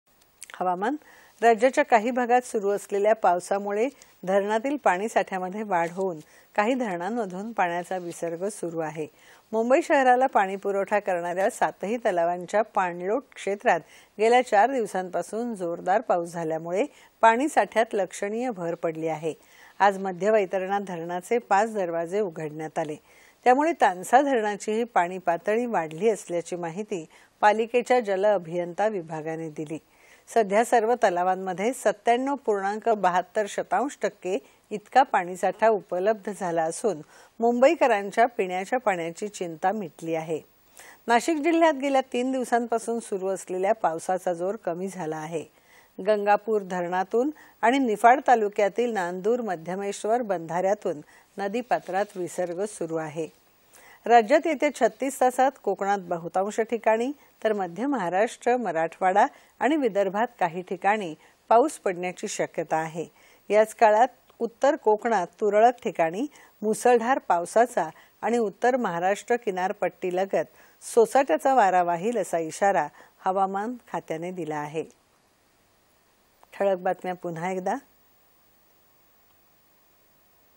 हवामान स्थिती
१४.०९.२०२१ :
    राज्याच्या काही  भागात सुरु असलेल्या पावसामुळे धरणातील पाणीसाठ्यामध्ये वाढ होऊन, काही धरणांमधून पाण्याचा विसर्ग सुरु  आहे.
   मुंबई शहराला पाणी पुरवठा करणाऱ्या  सातही तलावांच्या पाणलोट क्षेत्रात गेल्या चार दिवसापासून जोरदार पाऊस झाल्यामुळे  पाणी साठ्यात लक्षणीय भर पडली आहे. आज मध्य वैतरणा धरणाचे पाच दरवाजे उघडण्यात आले. त्यामुळे तानसा धरणाचीही पाणी पातळी वाढली असल्याची माहिती पालिकेच्या जलअभियंता विभागाने दिली. सध्या सर्व तलावांमध्ये ९७ पूर्णांक ७२ शतांश  टक्के इतका पाणीसाठा उपलब्ध झाला असून मुंबईकरांची पिण्याच्या पाण्याची चिंता मिटली आहे.
 नाशिक  जिल्ह्यात गेल्या तीन दिवसांपासून सुरू असलेला पावसाचा जोर कमी झाला आहे. गंगापूर धरणातून आणि  निफाड तालुक्यातील नांदूर-मध्यमेश्वर बंधाऱ्यातून  नदीपात्रात विसर्ग सुरू आहे.
    राज्यात येत्या ३६ तासात कोकणात बहुतांश ठिकाणी तर मध्य महाराष्ट्र , मराठवाडा आणि विदर्भात काही ठिकाणी पाऊस पडण्याची शक्यता आहे. याच काळात उत्तर कोकणात तुरळक ठिकाणी मुसळधार पावसाचा आणि उत्तर महाराष्ट्र किनार पट्टीलगत सोसाट्याचा वारा वाहील असा इशारा हवामान खात्याने दिला आहे.